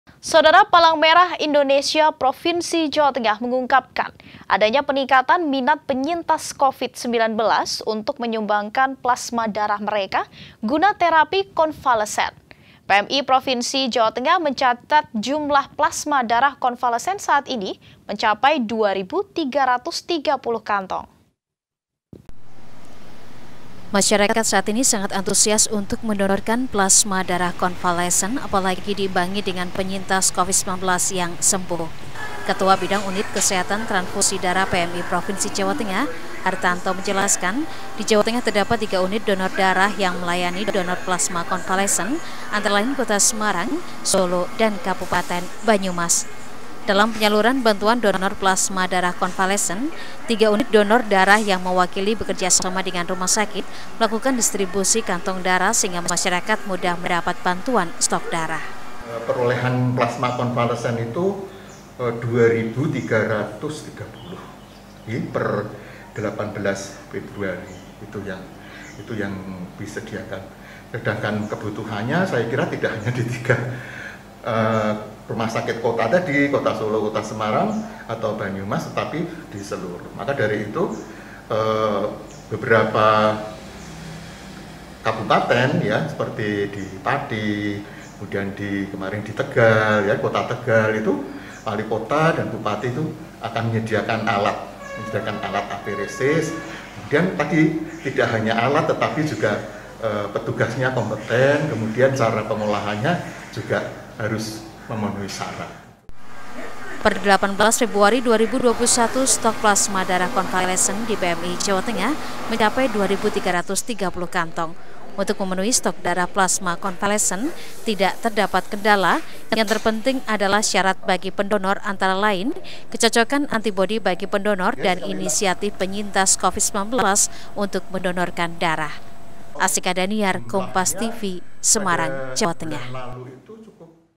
Stok Plasma Darah Konvalesen PMI Jateng Meningkat
SEMARANG, KOMPAS.TV - Palang Merah indonesia (PMI) Provinsi Jawa Tengah mengungkapkan adanya peningkatan minat penyintas Covid-19 untuk menyumbangkan plasma darah mereka, guna terapi konvalesen. PMI Provinsi Jawa Tengah mencatat jumlah plasma darah konvalesen saat ini mencapai 2.330 kantong.

Masyarakat saat ini sangat antusias untuk mendonorkan plasma darah konvalesen, apalagi diimbangi dengan penyintas Covid-19 yang sembuh. Ketua bidang unit kesehatan transfusi darah PMI Provinsi Jateng, Hartanto menjelaskan di Jawa Tengah terdapat 3 unit donor darah yang melayani donor plasma konvalesen antara lain Kota Semarang, Solo dan Kabupaten Banyumas. Dalam penyaluran bantuan donor plasma darah konvalesen, 3 unit donor darah yang mewakili bekerjasama dengan rumah sakit, melakukan distribusi kantong darah sehingga masyarakat mudah mendapat bantuan stok darah.

Per 18 Februari 2021 stok plasma darah konvalesen di PMI Jawa Tengah mencapai 2.330 kantong. Untuk memenuhi stok donor plasma konvalesen, tidak terdapat kendala yang terpenting adalah syarat bagi pendonor antara lain kecocokan antibodi bagi pendonor dan inisiatif penyintas Covid-19 untuk mendonorkan darah.